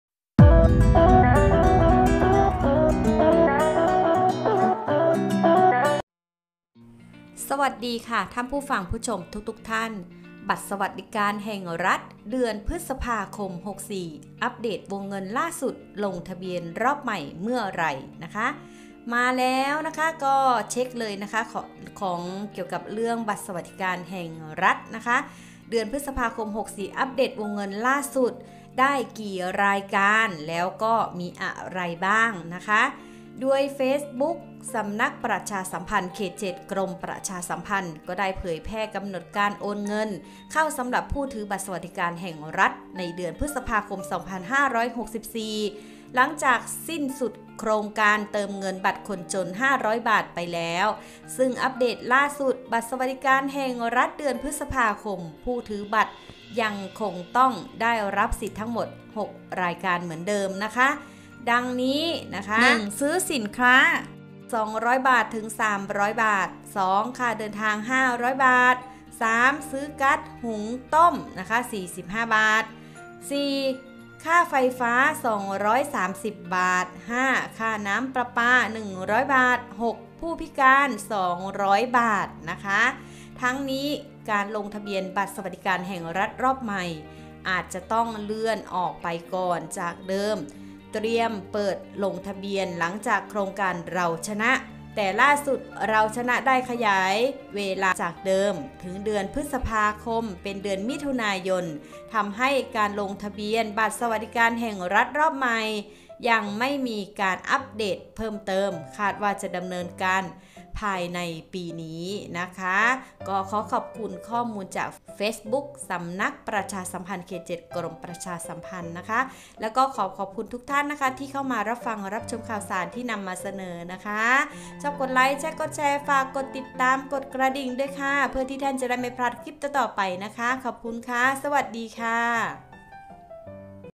บัตรสวัสดิการแห่งรัฐเดือนพฤษภาคม 61 วงเงินล่าสุดลงทะเบียนรอบใหม่เมื่อไร?
เช็คบัตรสวัสดิการแห่งรัฐเดือนพฤษภาคม 64 ล่าสุดได้กี่รายการอะไรบ้างโดยเขตประชาสัมพันธ์กรมประชาสัมพันธ์เขต7ได้เผยแพร่กำหนดการโอนเงินเข้าสำหรับผู้ถือบัตรสวัสดิการแห่งรัฐเดือนพฤษภาคม 64จากสิ้นสุดโครงการเติมเงินคนจน 500 บาทไปแล้วพึ่งอัพเดทล่าสุดบัตรสวัสดิการแห่งรัฐเดือนพฤษภาคมผู้ถือบัตรยังคงได้รับสิทธิ์ทั้งหมด 6 รายการเหมือนเดิม